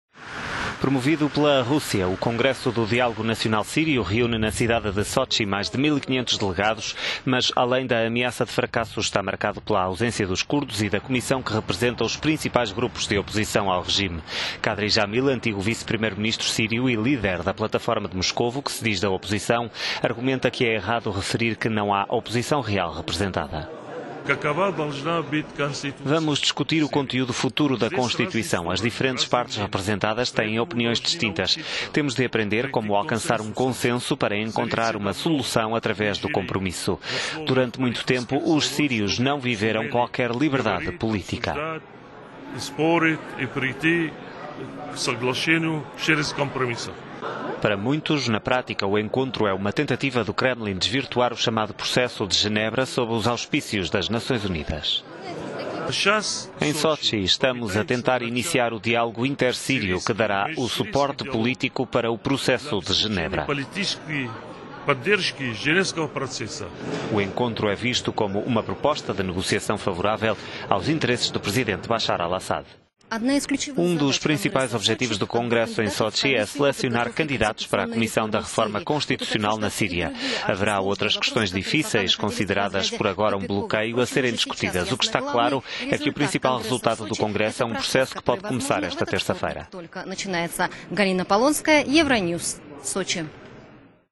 Reunião de Sochi ensombrada pelo vazio deixado pela oposição Síria
O Congresso do Diálogo Nacional Sírio procura encontrar uma solução política para o fim do conflito…